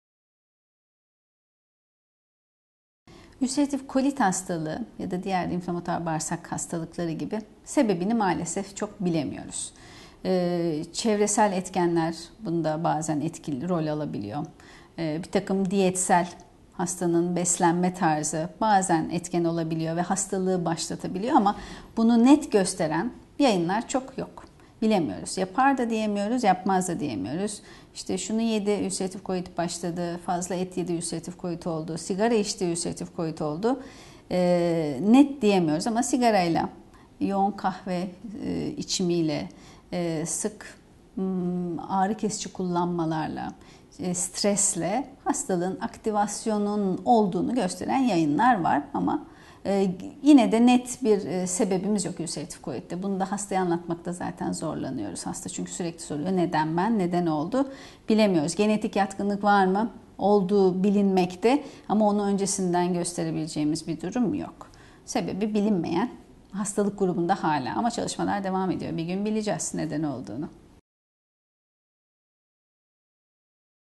Ülseratif Kolit hastalığının sebepleri nelerdir?
Acıbadem Fulya Hastanesi, İç Hastalıkları/Gastroenteroloji Uzmanı Dr. Özdal Ersoy, Bağırsak Hastalıkları üzerine soruları yanıtlıyor;